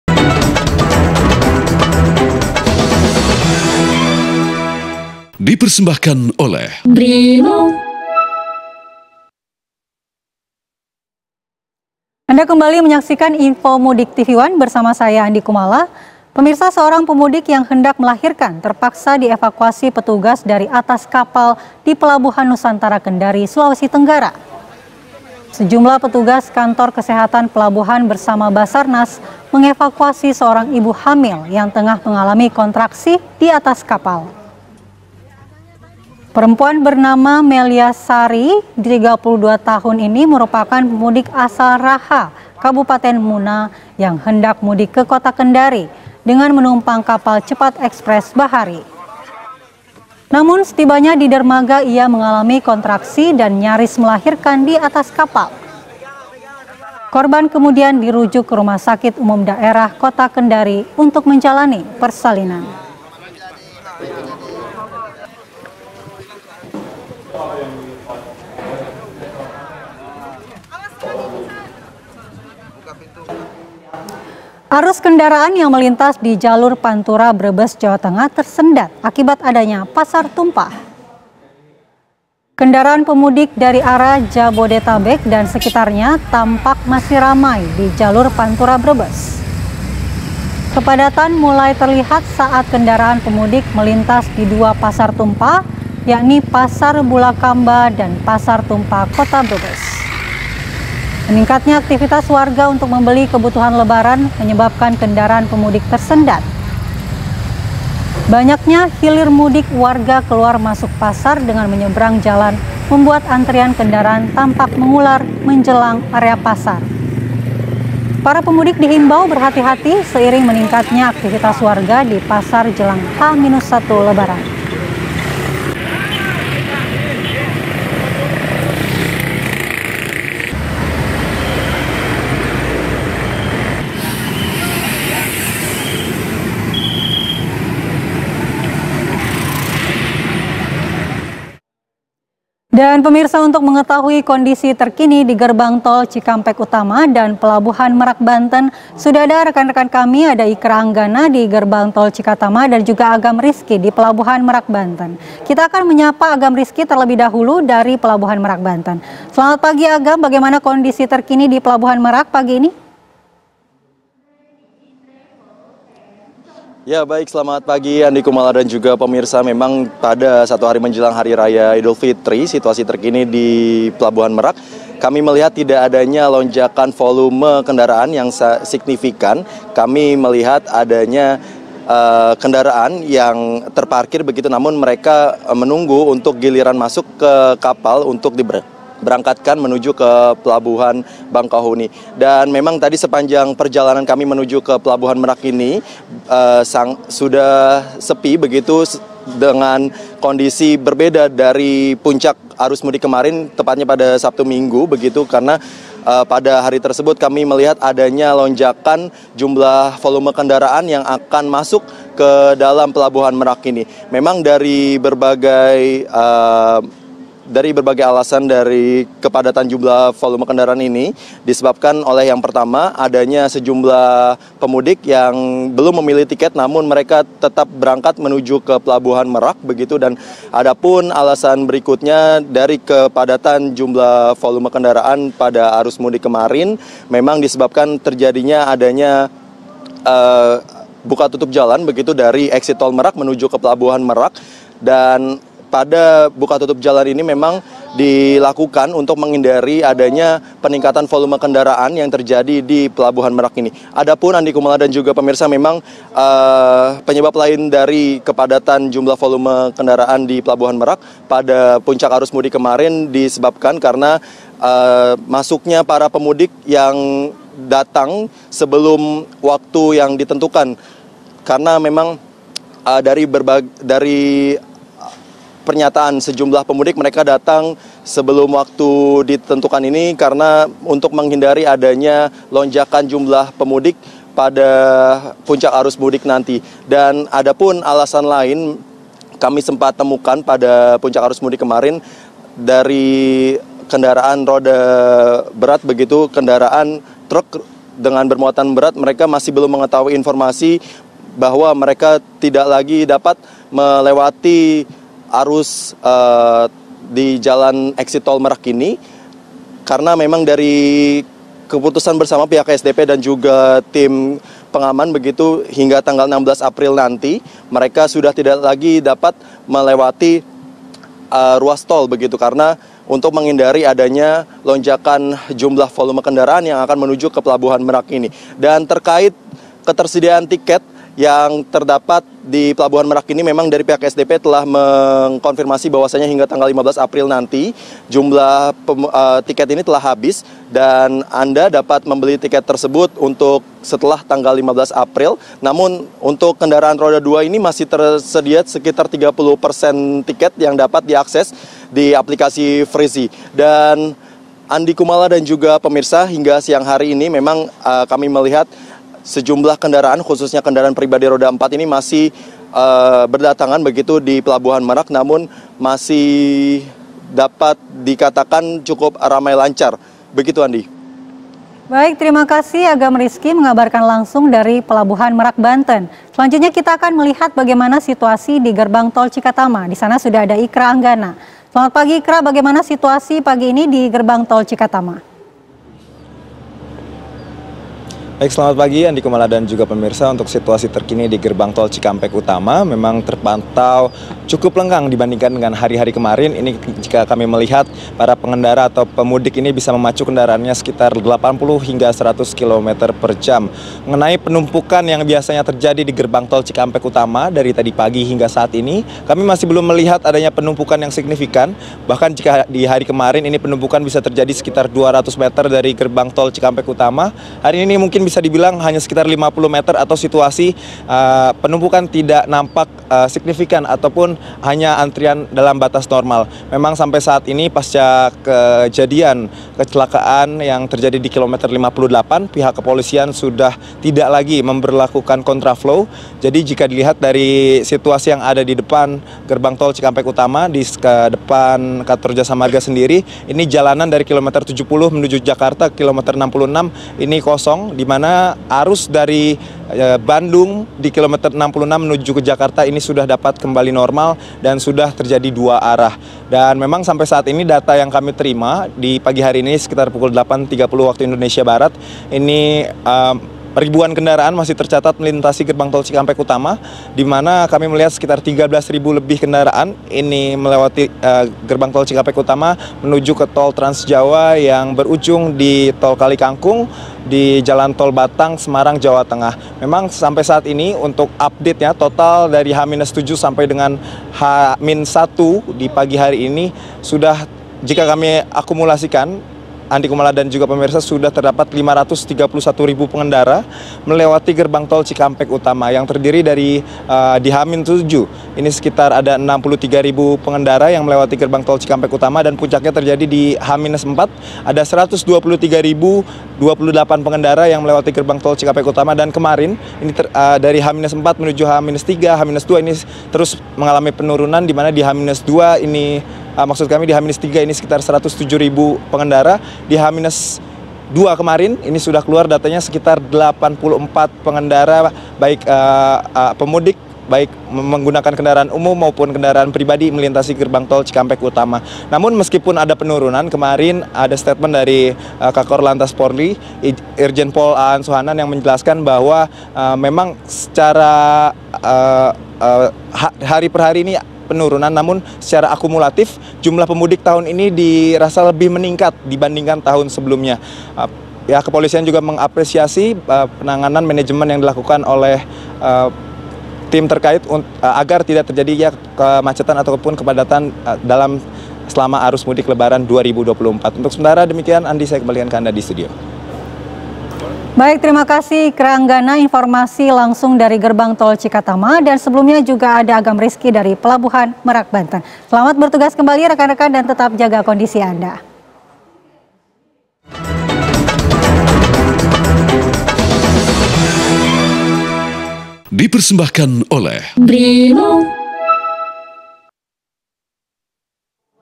Detik-detik Pemudik yang Hendak Melahirkan Terpaksa Dievakuasi dari Kapal | Info Mudik tvOne
Info Mudik, - Detik-detik Pemudik yang Hendak Melahirkan Terpaksa Dievakuasi dari Kapal | Info Mudik tvOne

Seorang wanita hamil yang tengah kontraksi dievakuasi oleh petugas dari atas kapal saat hendak melakukan mudik lebaran di Pelabuhan Nusantara, Kota Kendari, Sulawesi Tenggara, Senin, 8 April 2024.

MUDIK01
FR01
AML01